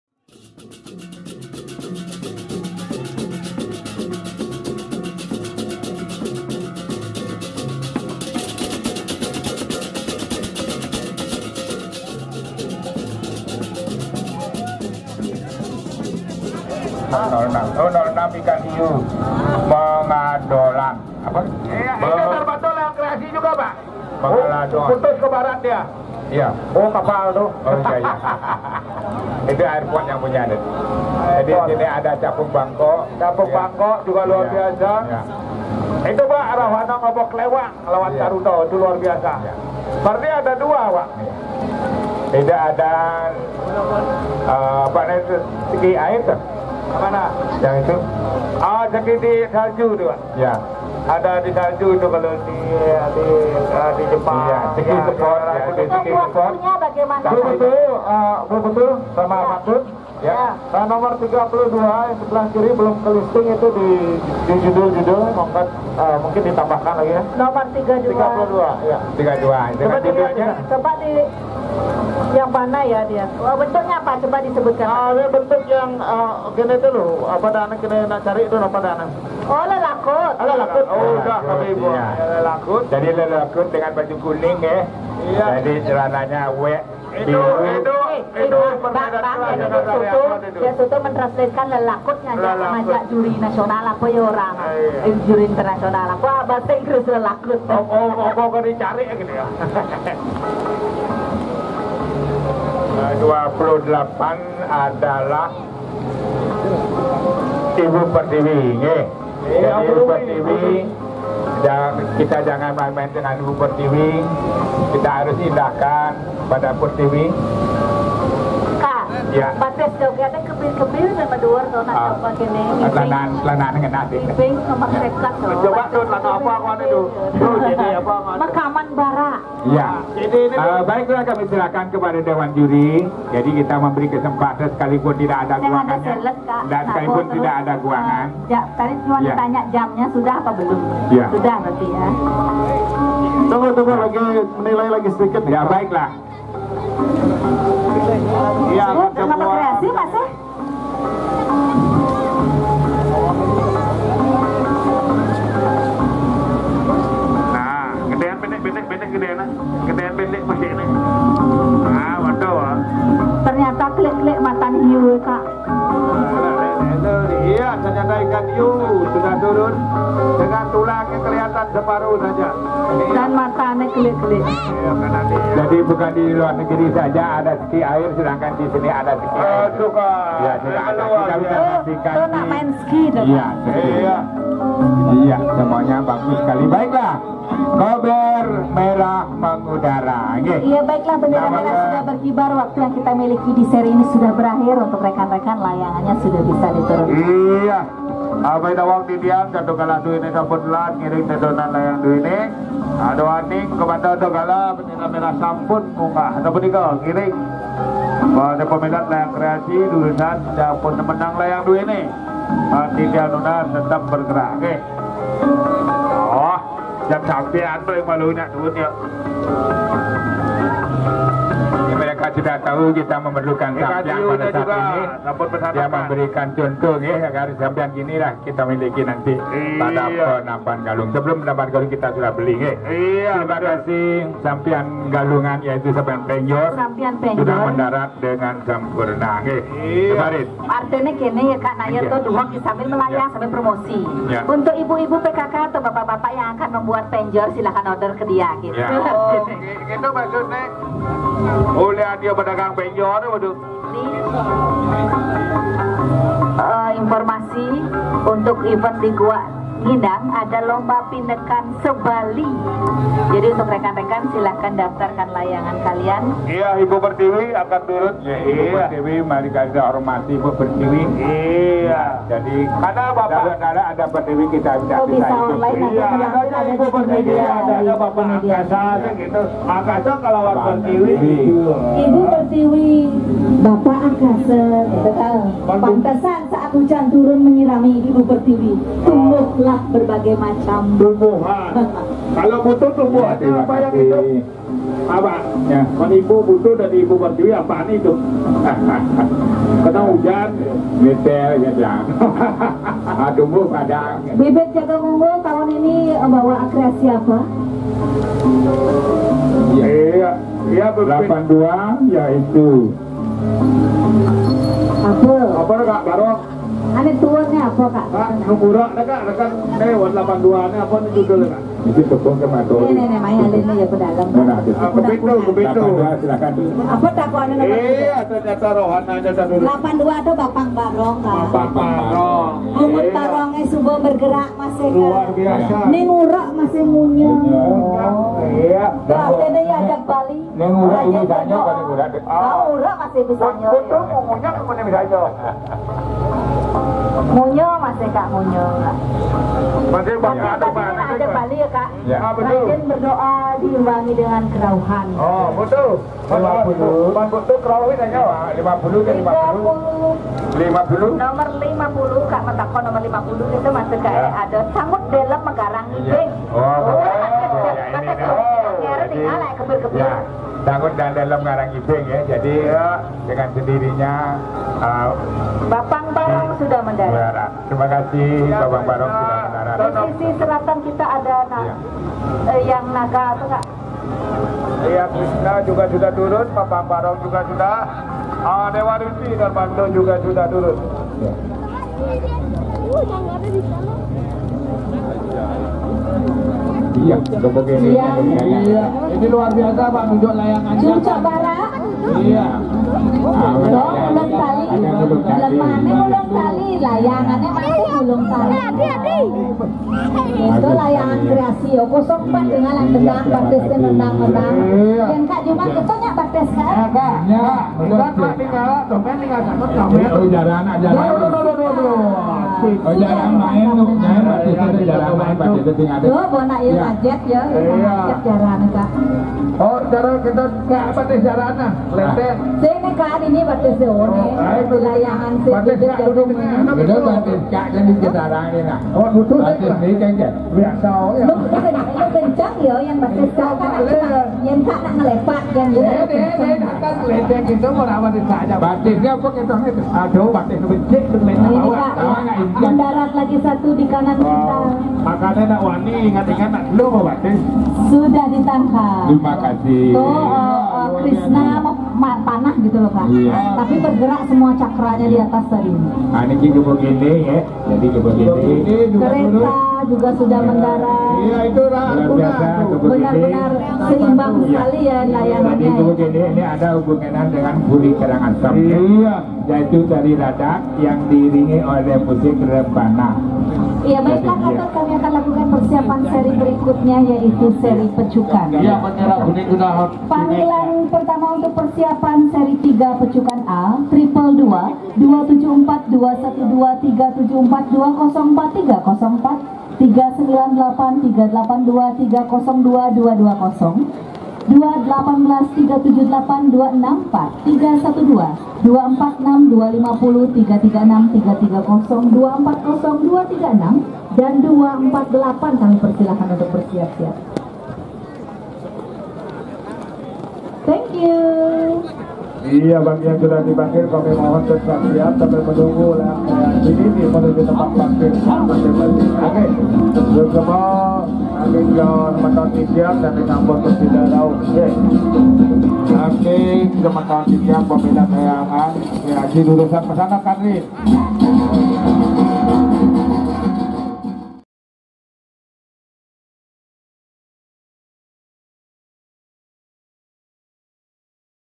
LAYANGAN KREASI BARU 2023 HIASI LANGIT PADANGGALAK
Pelangi Bali 2023.
Pelangi Bali Kite Festival 2023.
Opening Bali Kite Festival 2023.
Pantai Padanggalak, 16 Juli 2023.
Seri Layangan Kreasi Baru